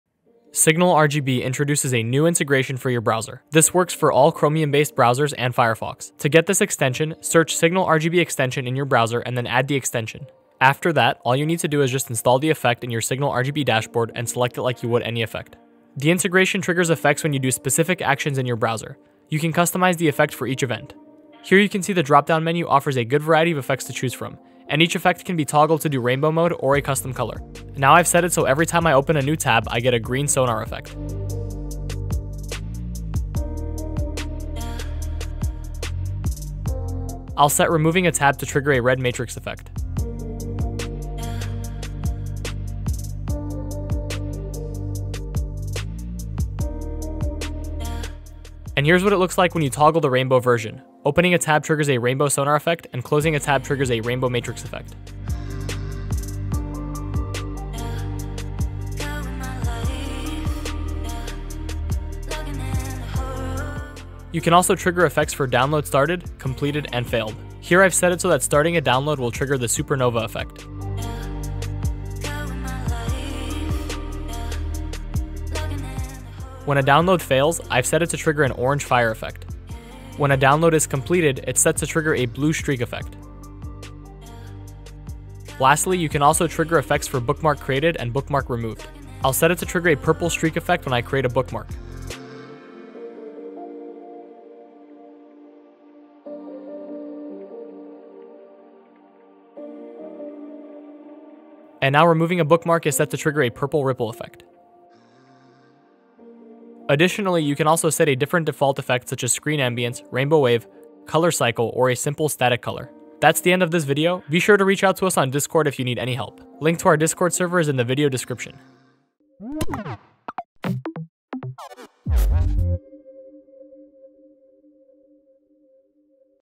Make Your RGB Setup React With Chrome, Opera GX, And More! | SignalRGB
This is the SignalRGB Browser Integration. An RGB integration links your RGB gaming setup usually with games but this time it's with your browser. When a specific event happens in your browser then your RGB setup will flash particular colors depending on the browser event. This is compatible with almost every browser including Chrome, Brave, Firefox, Opera GX, and more!